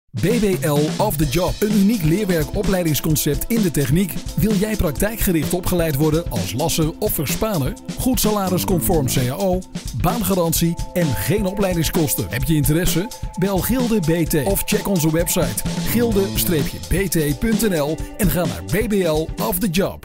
BBL off the Job commercial
BBL off the job een uniek leerwerk opleidingsconcept in de techniek!
Wil jij praktijkgericht opgeleid worden als lasser of verspaner?
Wil jij werken bij een toonaangevende industriële bedrijven in de regio
Wij hebben de job voor jou!
Goed salaris conform cao/ Geen opleidingskosten/ Baan garantie
Heb je interesse bel Gilde-BT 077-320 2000 of check onze webiste en ga naar BBL off the job